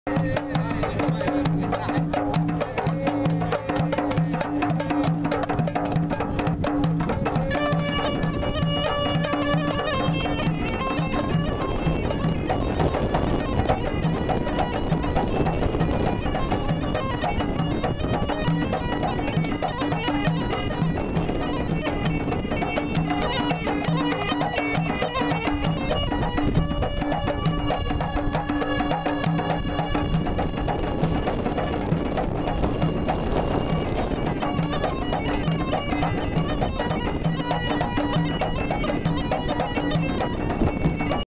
La Goulette Tunis Cruise port
Musicians playing at dockside. La Goulette has great facilities for cruise passengers.